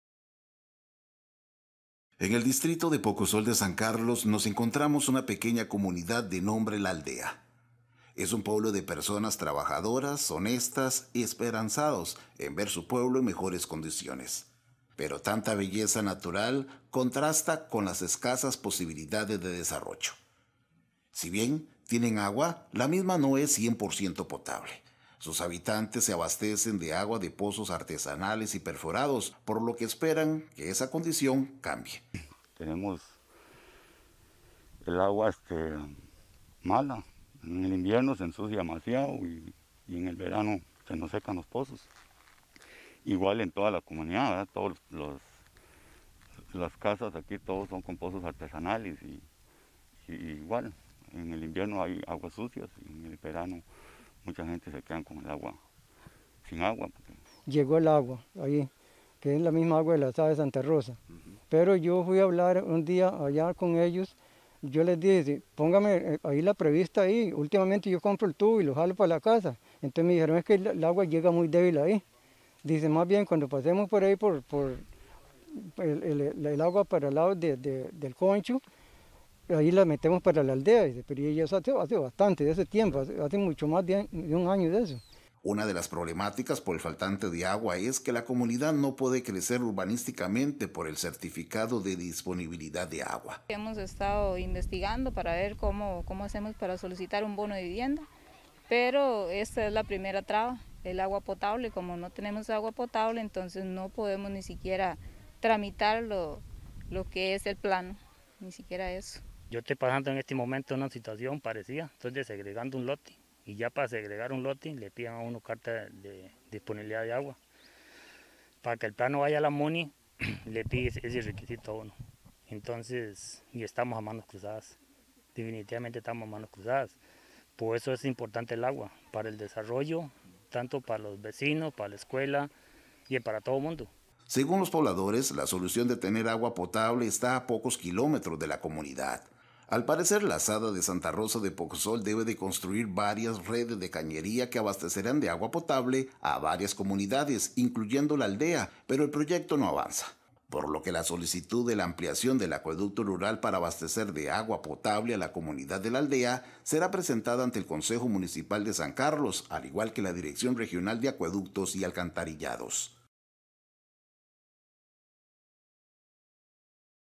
POBLADORES DE LA ALDEA PIDEN A LA ASADA DE SANTA ROSA AGUA POTABLE
Los habitantes aseguran que la solución está a pocos kilómetros.